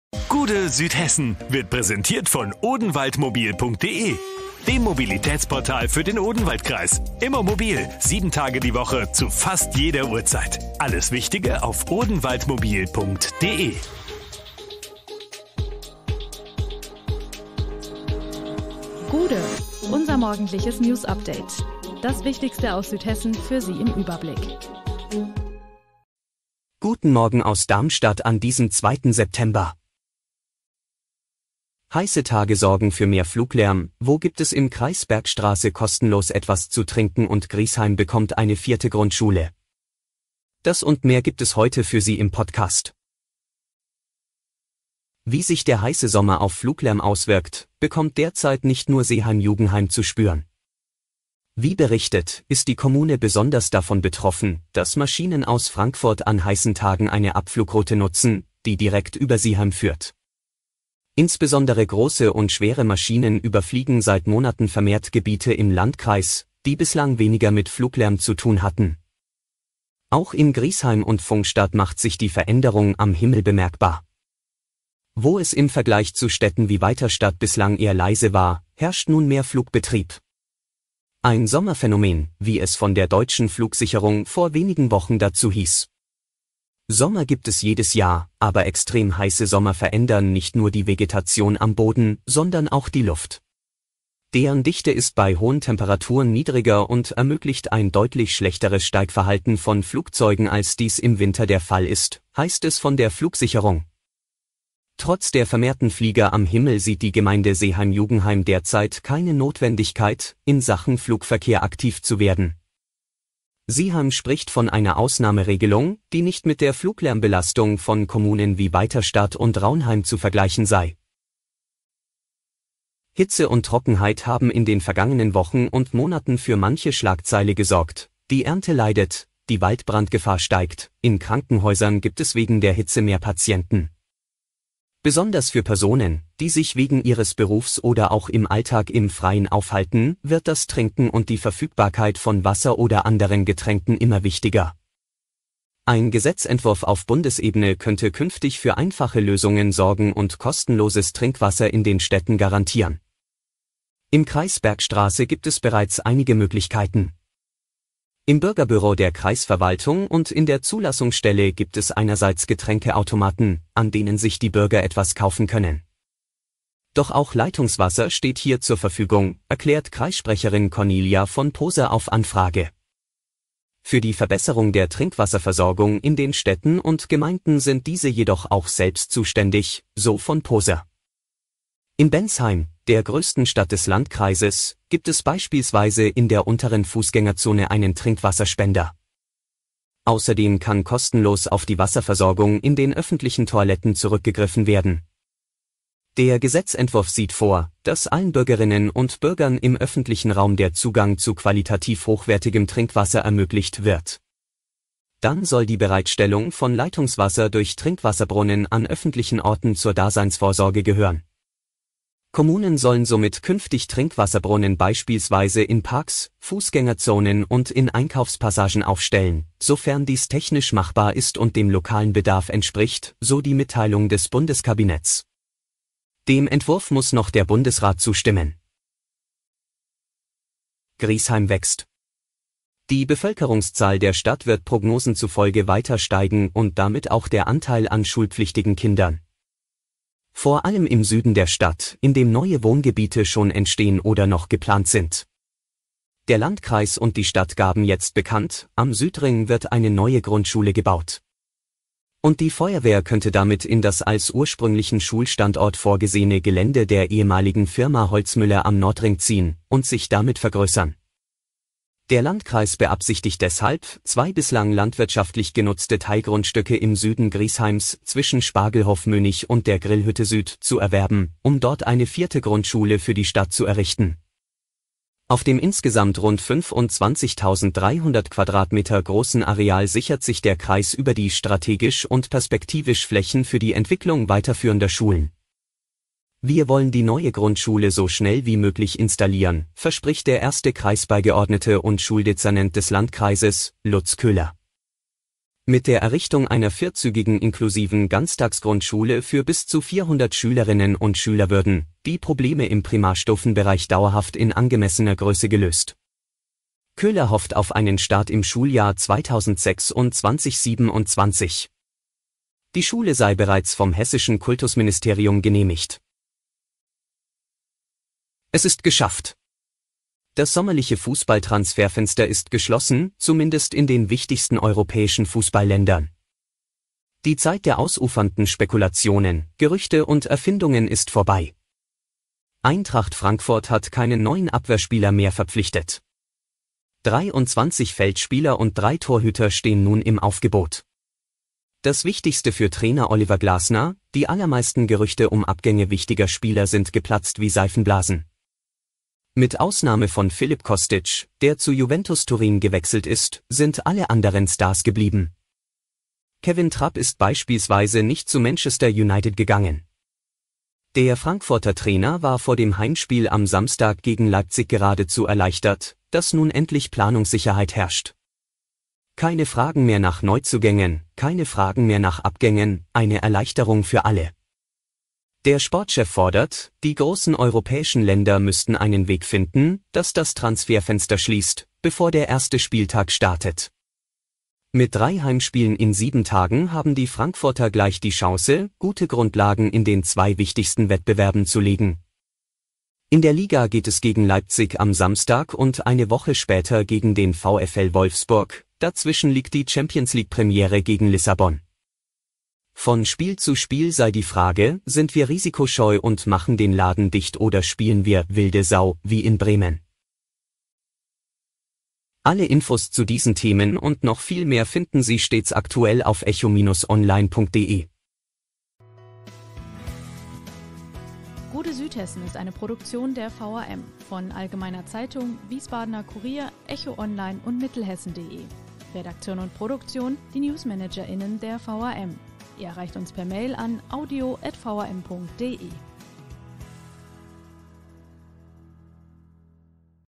Heiße Tage sorgen für mehr Fluglärm - Gude, Südhessen!
Heiße Tage sorgen für mehr Fluglärm, wo gibt es im Kreis Bergstraße kostenlos etwas zu trinken und Griesheim bekommt eine vierte Grundschule. Das und mehr gibt es heute für Sie im Podcast. Alle Hintergründe zu den Nachrichten des Tages finden Sie hier: